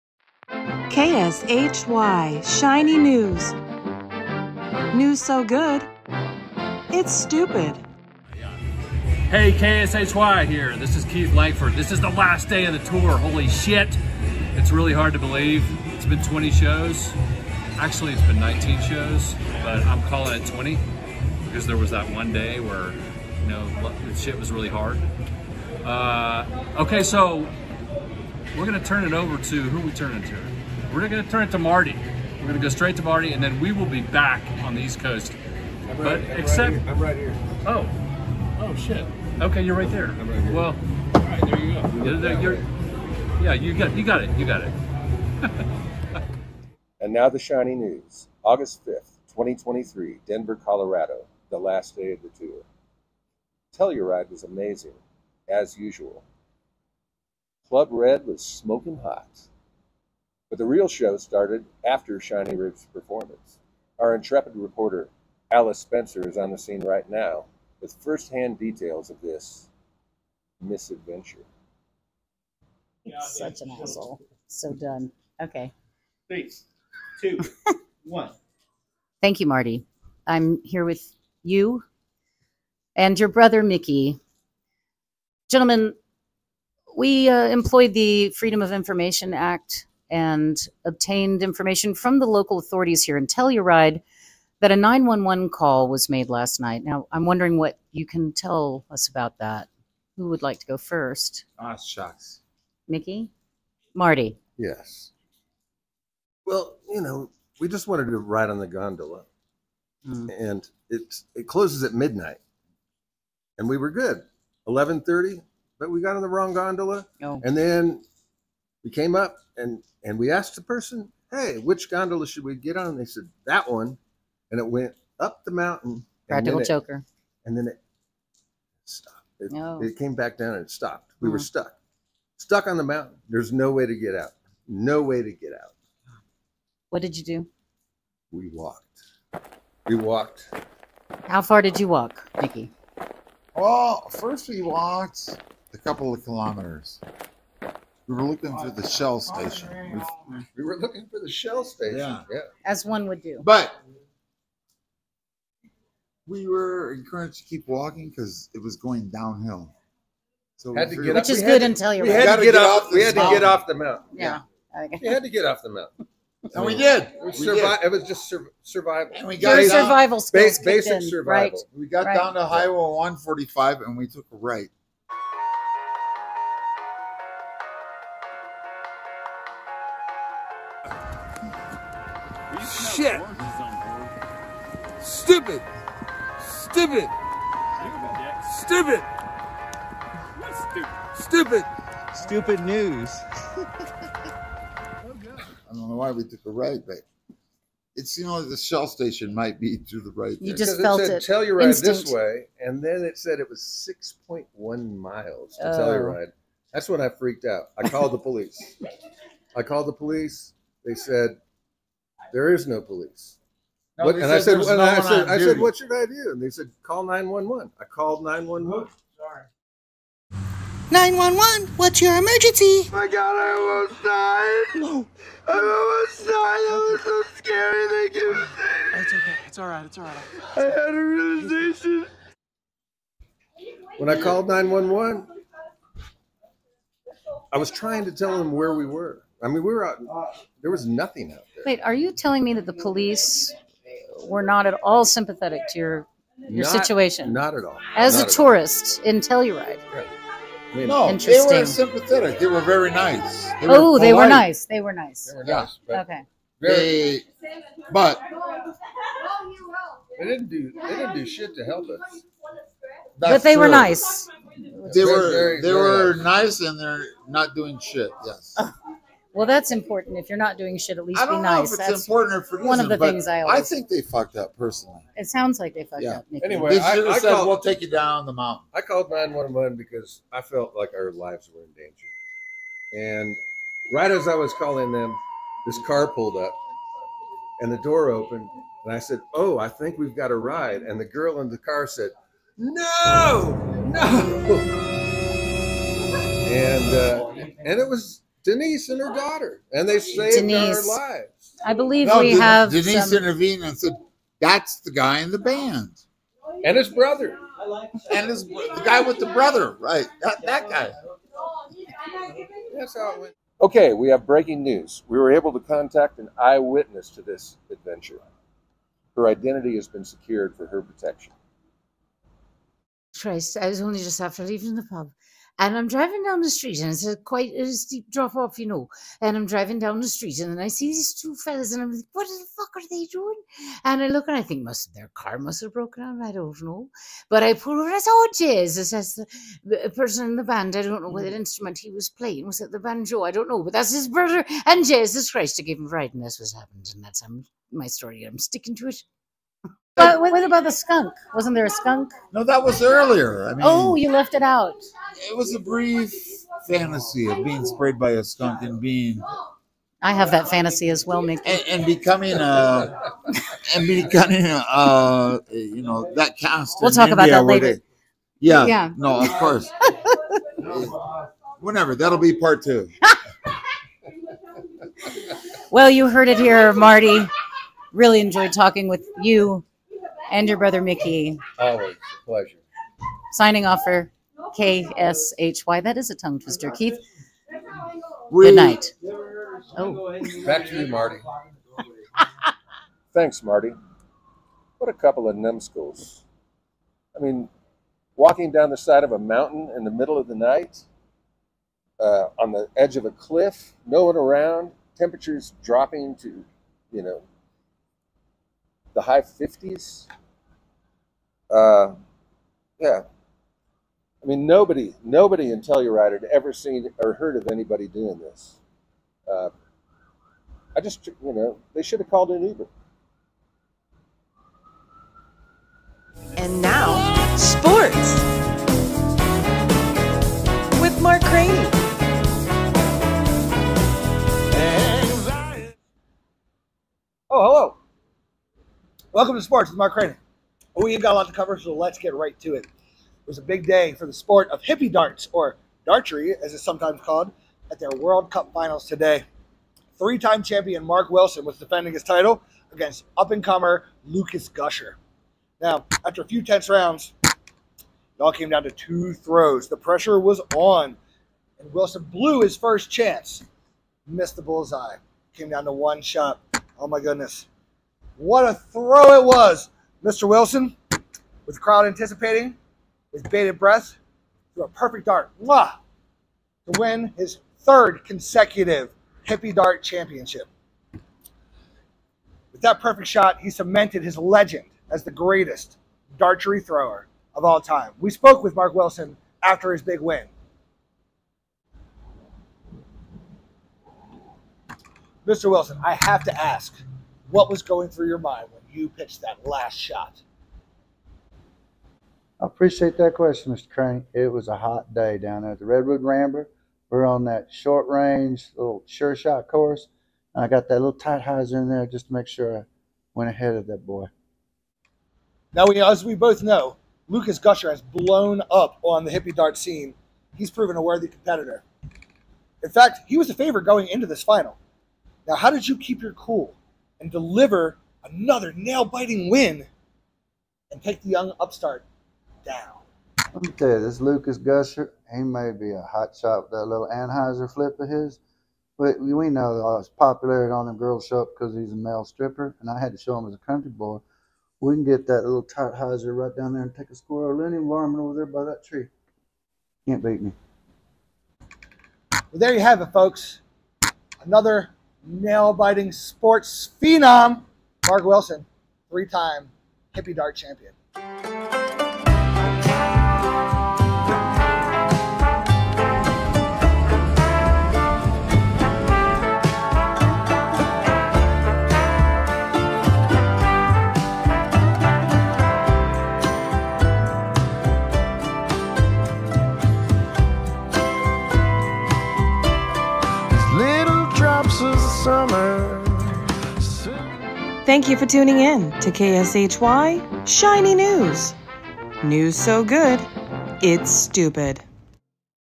SHINY NEWS VOL10
We've just wrapped up our West Coast tour and everyone was amazing! Even the police were nice... Why did Marty need to call 911? Watch to find out this and more!